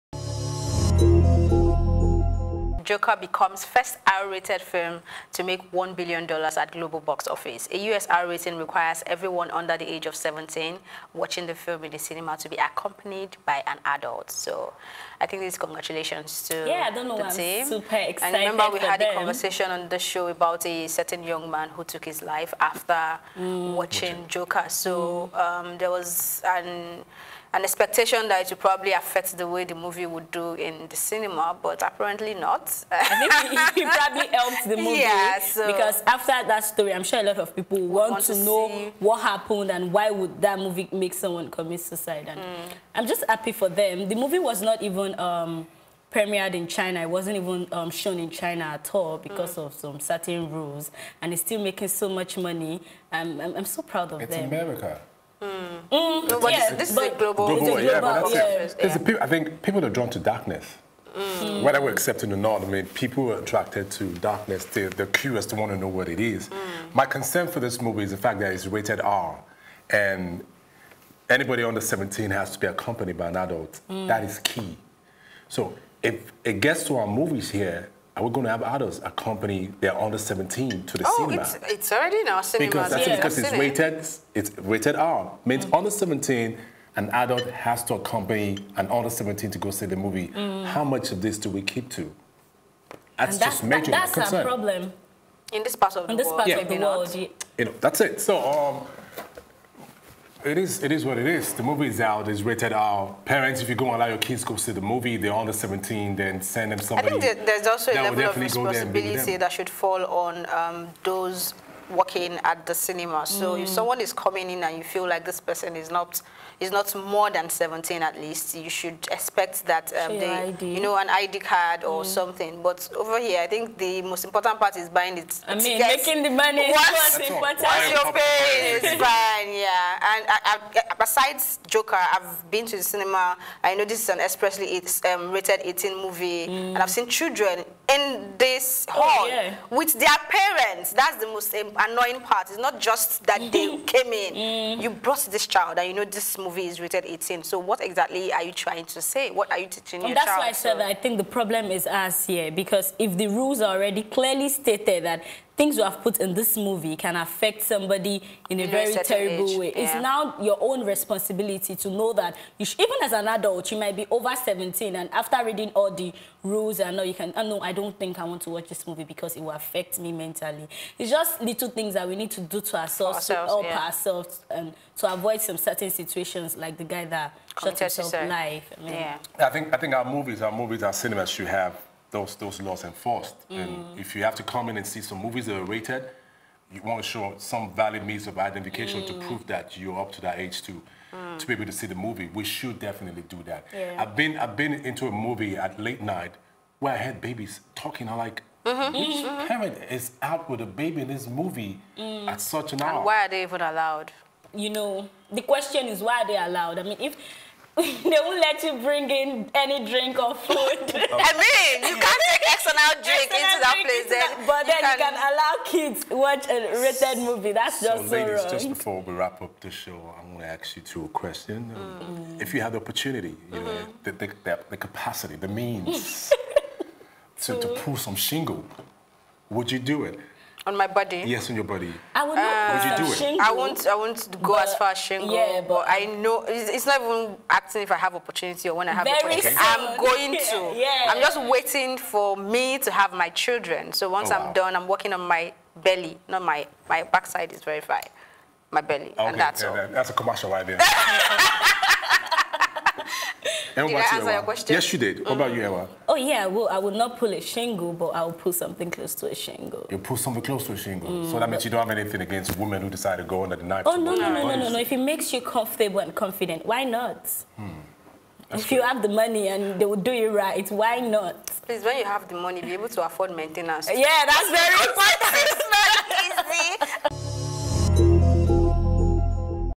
Joker Becomes First R-Rated Movie To Hit $1bn At Global Box Office
Joker becomes first R-rated film to make $1bn at global box office